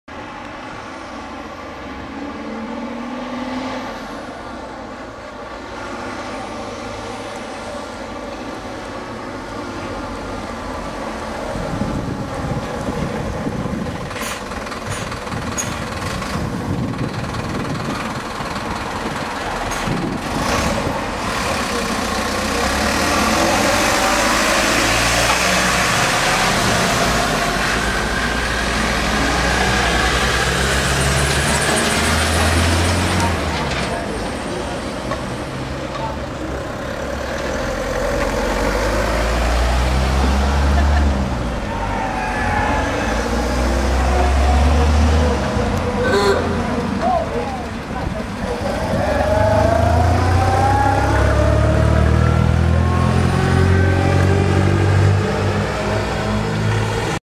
Leaving Cumbria steam gathering 2022
Mercedes beavertail truck then vintage car right at the end